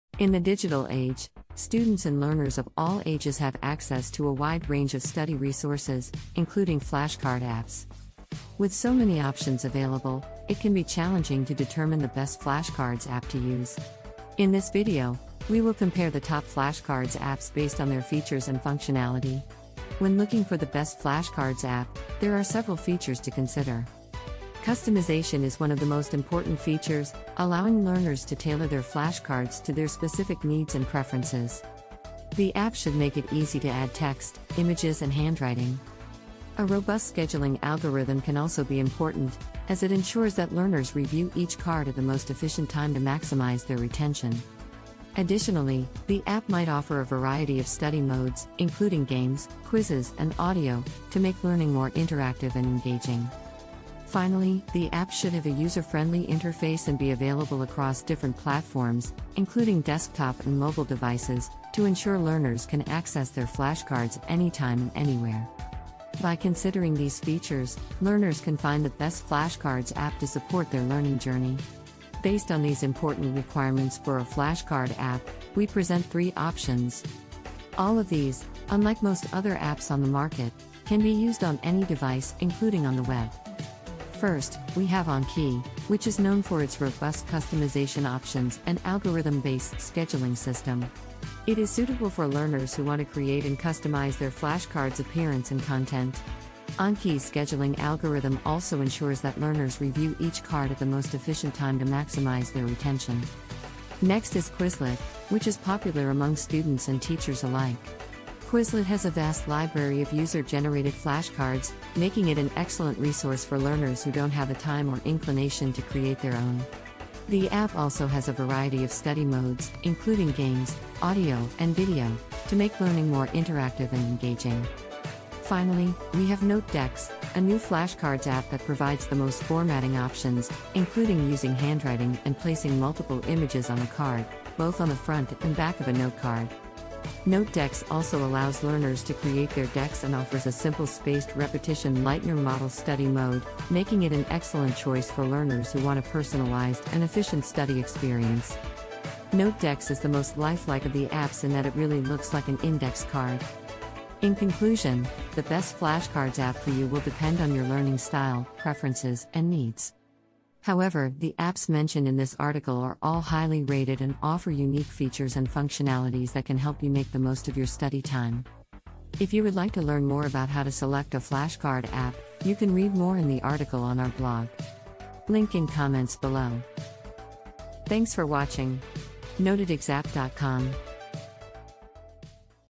The Best Flashcard Apps that work on Any Device!
In the digital age, students and learners of all ages have access to a wide range of study resources, including flashcard apps.

With so many options available, it can be challenging to determine the best flashcards app to use. In this video, we will compare the top flashcards apps based on their features and functionality.

When looking for the best flashcards app, there are several features to consider. Customization is one of the most important features, allowing learners to tailor their flashcards to their specific needs and preferences. The app should make it easy to add text, images and handwriting. A robust scheduling algorithm can also be important, as it ensures that learners review each card at the most efficient time to maximize their retention. Additionally, the app might offer a variety of study modes, including games, quizzes, and audio, to make learning more interactive and engaging. Finally, the app should have a user-friendly interface and be available across different platforms, including desktop and mobile devices, to ensure learners can access their flashcards anytime and anywhere. By considering these features, learners can find the best flashcards app to support their learning journey.

Based on these important requirements for a flashcard app, we present 3 options. All of these, unlike most other apps on the market, can be used on any device, including on the web.

First, we have Anki, which is known for its robust customization options and algorithm-based scheduling system. It is suitable for learners who want to create and customize their flashcards' appearance and content. Anki's scheduling algorithm also ensures that learners review each card at the most efficient time to maximize their retention.

Next is Quizlet, which is popular among students and teachers alike. Quizlet has a vast library of user-generated flashcards, making it an excellent resource for learners who don't have the time or inclination to create their own. The app also has a variety of study modes, including games, audio, and video, to make learning more interactive and engaging.

Finally, we have NoteDex, a new flashcards app that provides the most formatting options - including using handwriting and placing multiple images on a card - both on the front and back of a notecard. NoteDex also allows learners to create their decks and offers a simple spaced repetition Leitner model study mode, making it an excellent choice for learners who want a personalized and efficient study experience. NoteDex is the most life-like of the apps in that it really looks like an index card.

In conclusion, the best flashcards app for you will depend on your learning style, preferences, and needs. However, the apps mentioned in this article are all highly rated and offer unique features and functionalities that can help you make the most of your study time. If you would like to learn more about how to select a flashcard app, you can read more in the article on our blog. Link in comments below.